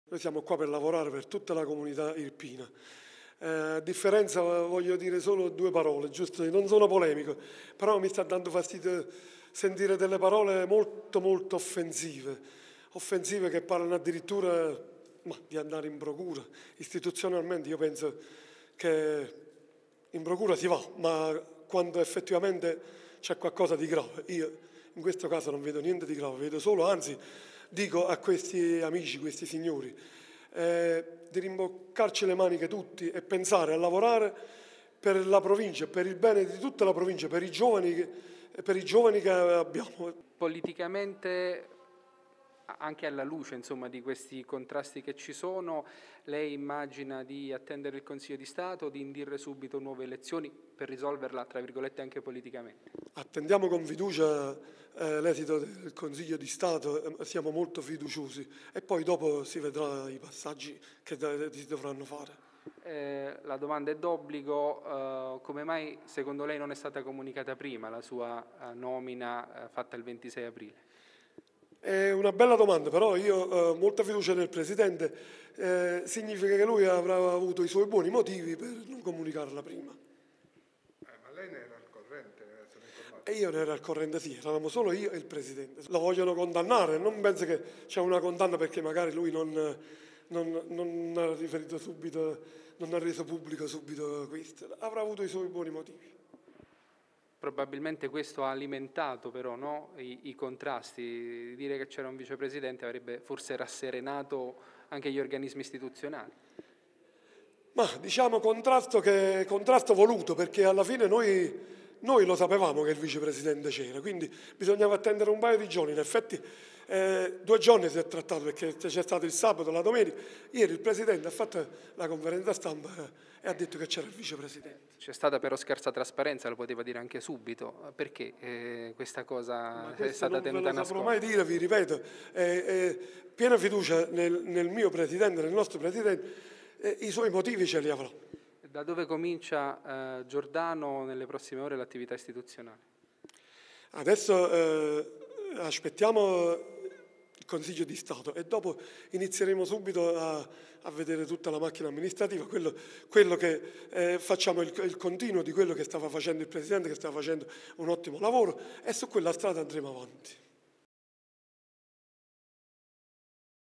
Provincia. Giordano vicepresidente: «Prima di nuove elezioni aspettiamo il Consiglio di Stato»
Primo giorno in Provincia da reggente per il sindaco di Monteforte irpino nominato lo scorso 26 aprile. «Perché Buonopane ha tenuto nascosta la mia nomina? Avrà avuto i suoi buoni motivi. Non ci sono irregolarità. Aspettiamo il Consiglio di Stato prima di nuove votazioni. Avanti nel solco della continuità»»